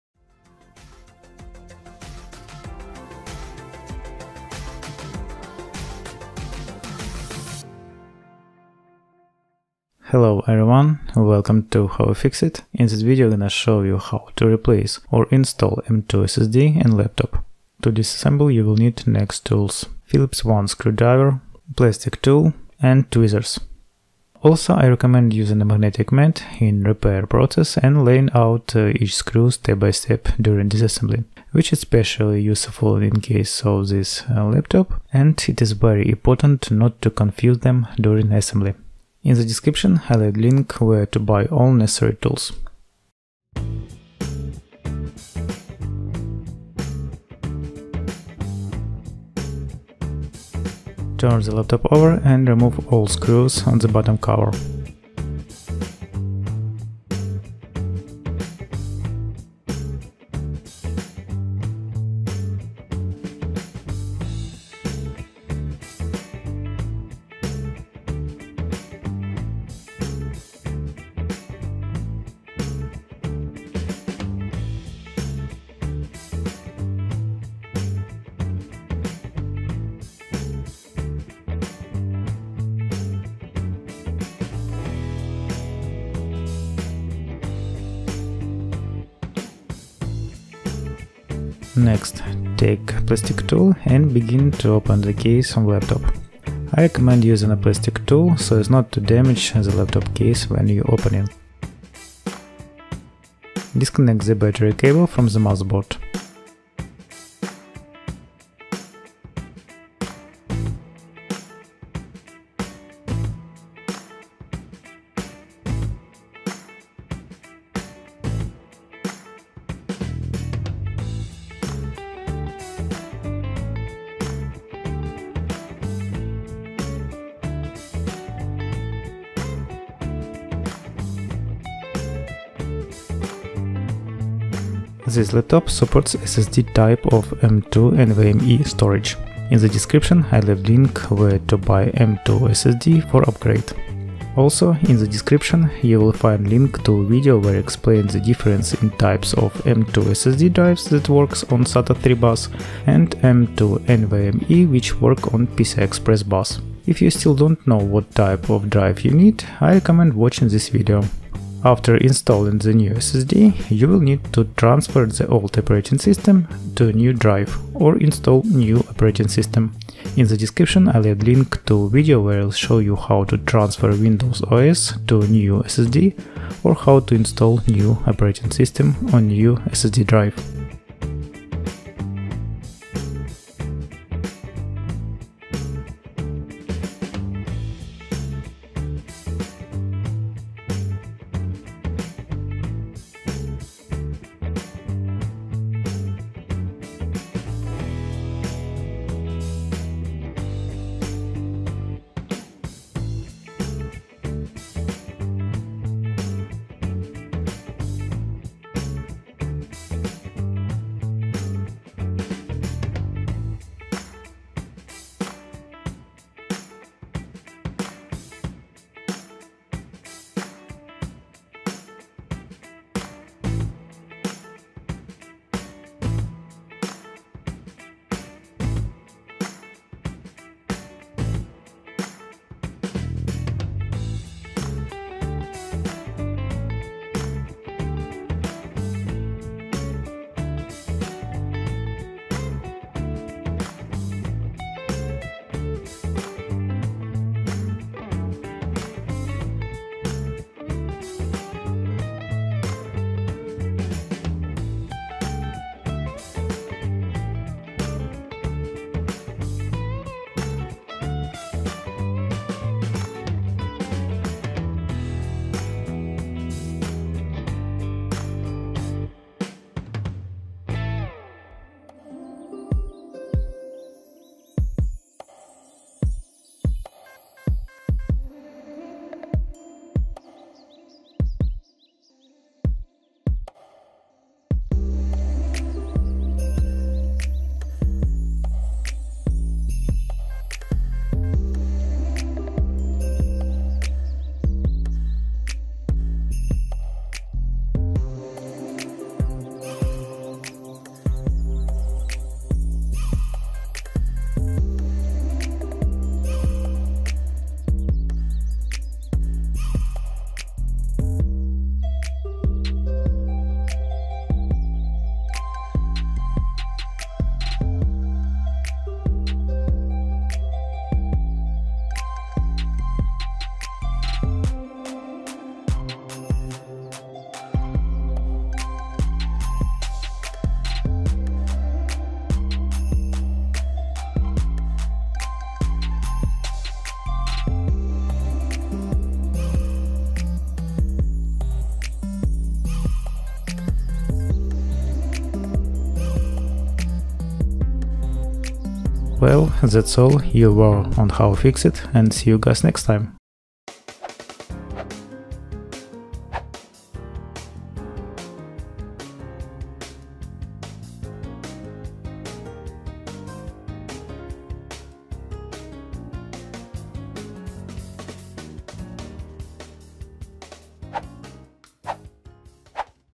HP Spectre x360 13-ap M2 SSD Upgrade - DIY Installation Guide
This step-by-step tutorial will help you upgrade and install M2 SSD in HP Spectre x360 13-ap, 13-ap0013dx, 13-ap0023dx, 13-ap0033dx laptop for better speed and performance.

*DESCRIPTION*
In this guide, I'll show you how to upgrade and install an M2 SSD on HP Spectre x360 13-ap, 13-ap0013dx, 13-ap0023dx, 13-ap0033dx models. This simple step-by-step tutorial will help you replace your existing drive with an M2 NVMe SSD for better performance.

⚠ The HP Spectre 13-ap0xxx series (which includes models like 13-ap0013dx, 13-ap0023dx, 13-ap0033dx, etc.) is typically referred to as the “Gem-Cut” Spectre 13. These laptops are designed with a PCIe NVMe-only M.2 slot. In other words, an M.2 SATA drive will not be recognized.
*Key Points*
✔️ PCIe NVMe (Gen 3 x4 or x2 depending on exact model).
✔️ No SATA (AHCI) support on the M.2 slot.
✔️ M.2 2280 (80 mm length).
✔️ The laptop is extremely thin, leaving no space for a 2.5‑inch drive.
If you have an HP Spectre 13-ap0xxx model, be sure to use an M.2 NVMe (PCIe) SSD when upgrading or replacing the internal storage. M.2 SATA drives are not compatible with this generation of the Spectre 13.

If you need help choosing between M.2 SATA and M.2 NVMe drives, check out the Explaining Storage Devices video for a detailed comparison. Once the new drive is installed, you can either clone your operating system or perform a clean Windows OS installation. We’ve provided a step-by-step guide in another linked video to walk you through both methods—check the Useful Links section.

💡 What You'll Learn:
✅ How to disassemble the HP Spectre x360 13-ap laptop
✅ How to safely remove and replace the M.2 SSD
✅ Essential tools for this upgrade
✅ Reassembling the HP Spectre x360 13-ap laptop
✅ Tips for choosing the right storage device

Question: What is the maximum M2 SSD size for the HP Spectre x360 13-ap laptop?
Answer: There is no limit on the maximum size of M2 SSD, but there are Windows limitations. If your Windows is 64-bit and the GPT file system - you can install a drive of any size.

Question: What should I do after installing a new M2 SSD?
Answer: You can clone your existing OS or do a fresh Windows installation —watch the linked video for details.

Question: Do I need to reinstall Windows after upgrading my SSD?
Answer: You can either clone your OS or install Windows from scratch. Watch the linked tutorial for details.

Question: My laptop doesn’t boot from the SSD. What should I do?
Answer: Go to BIOS settings and ensure the SSD is set as the primary boot device.